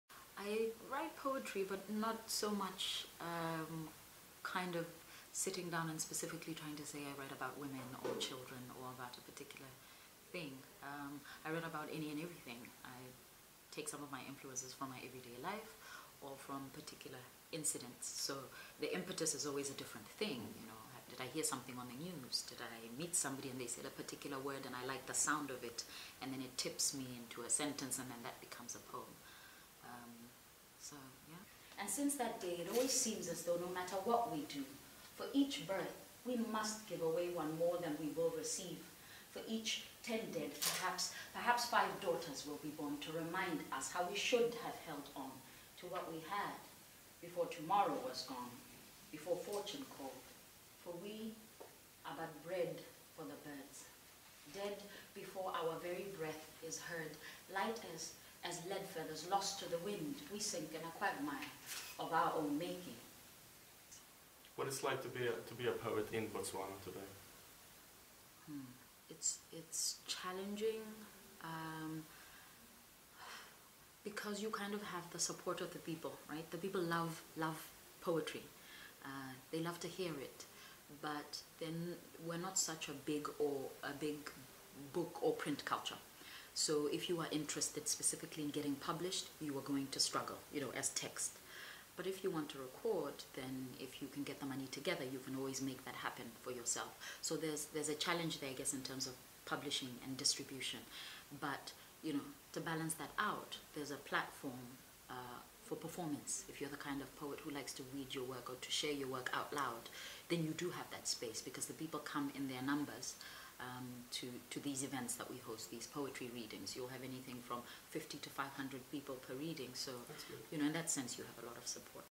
Botswana poets at NAI: TJ Dema
In October 2014, four poets from Botswana visited Uppsala and participated in Ordsprak Uppsala International Poetry Festival. They also came to NAI for a performance.